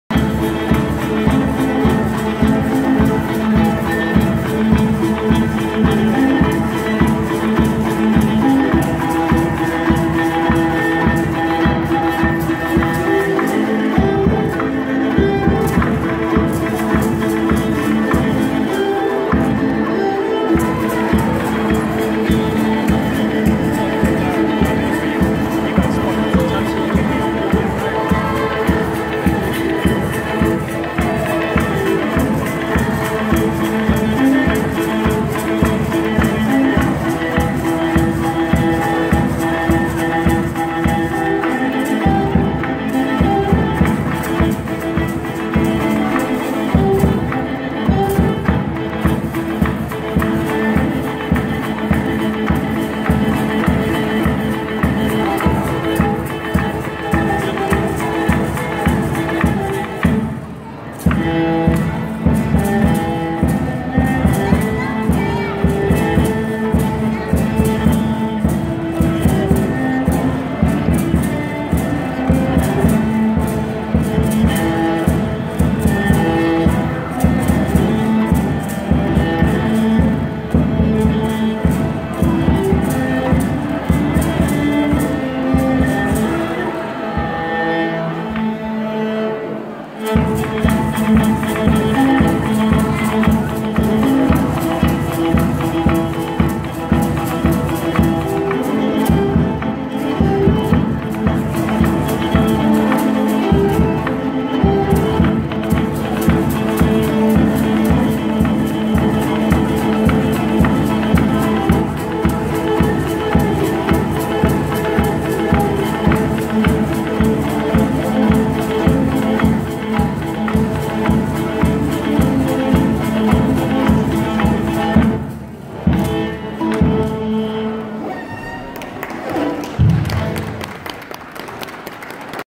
English School of Mongolia, Christmas fair, 26 Nov 2016
A music ensemble comprising morin khuurs aka horsehead fiddles + drum(s), performing at the Christmas fair at the English School of Mongolia, in the capital Ulaanbaatar on 26 Nov 2016.  I was working there for 6 weeks during 23 Oct - 2nd Dec.